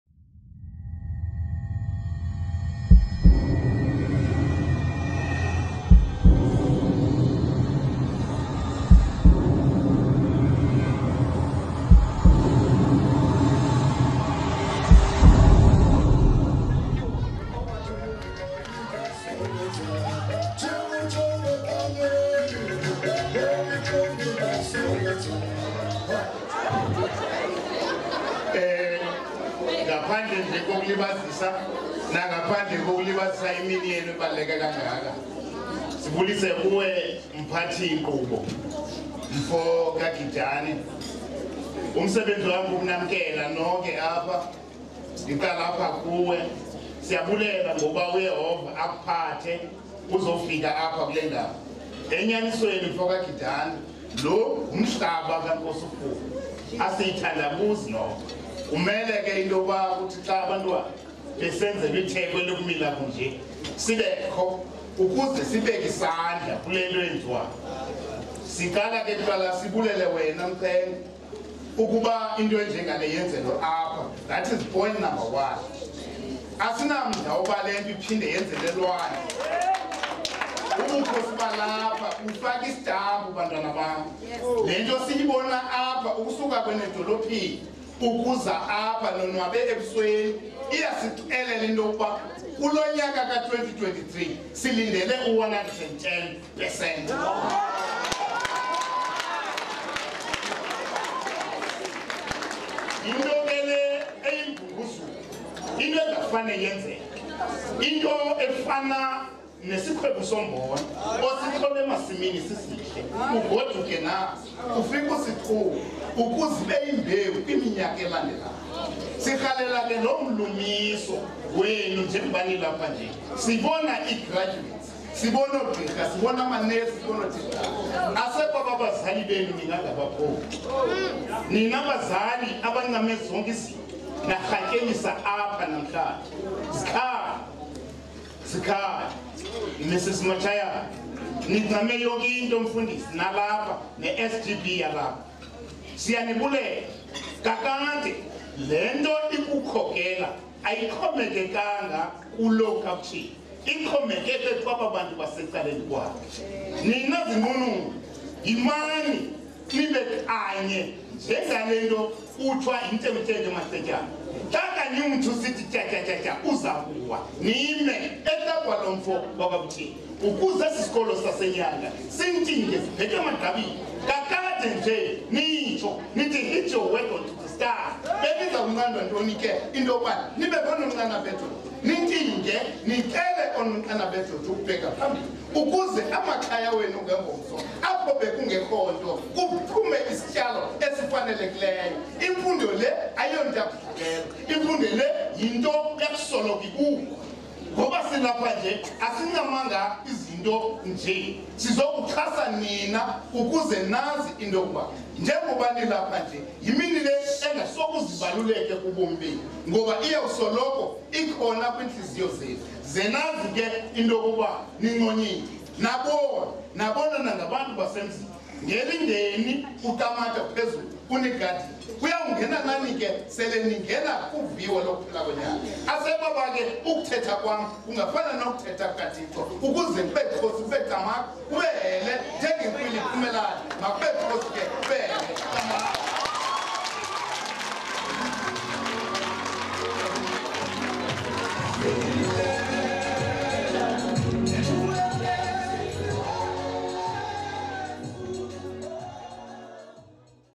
Welcoming by Prince Dalasile
Prince Dalasile welcoming everybody at Nyanga High G12 2023 farewell eNgcobo, Eastern Cape, South Africa.